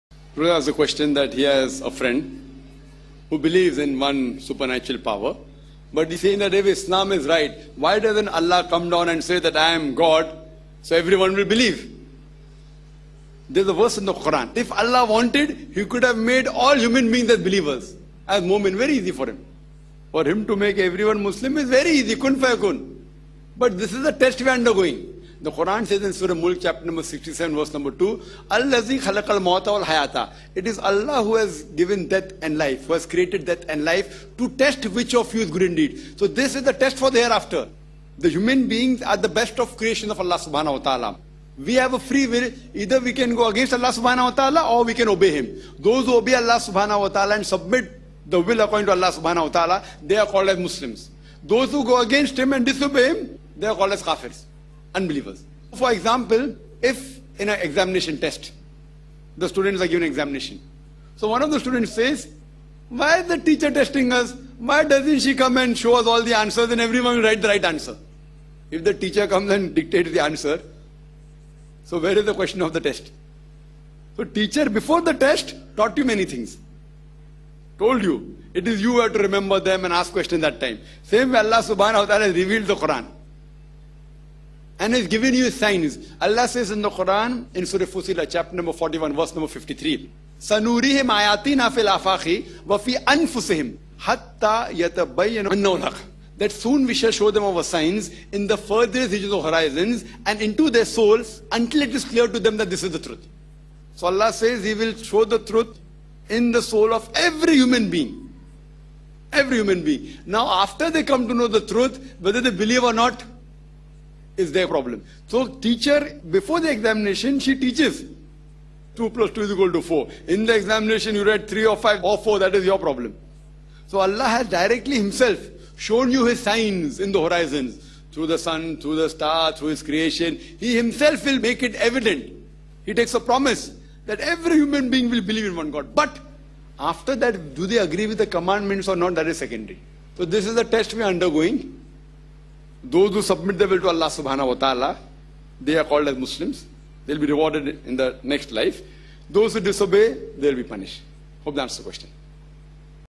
Dr. Zakir Naik - Why don't Allah (God) come down on earth?
Why don't Allah (God) come down on earth? answer by Dr. Zakir Naik.